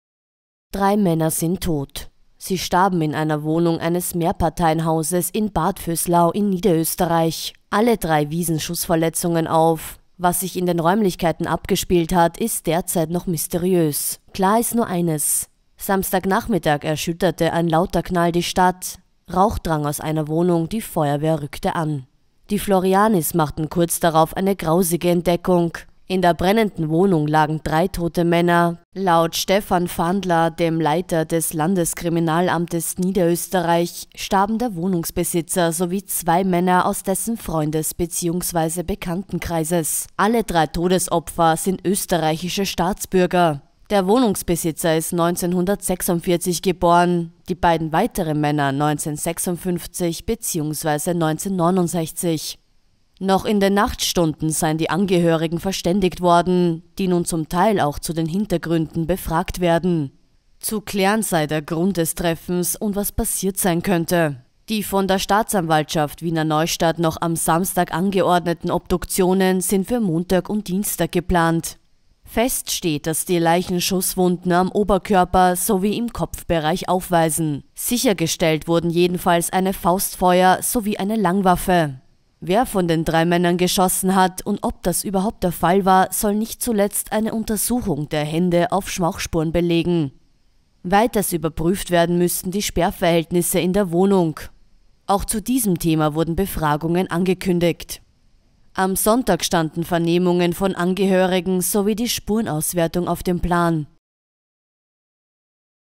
Drei Tote in Bad Vöslau: Identitäten stehen fest | krone.tv NEWS
Nach dem Fund von drei Toten mit Schusswunden in Bad Vöslau (NÖ) am Samstagnachmittag stehen die Identitäten fest. Laut Stefan Pfandler, dem Leiter des Landeskriminalamts Niederösterreich, starben der Wohnungsbesitzer sowie zwei Männer aus dessen Freundes- bzw. Bekanntenkreis.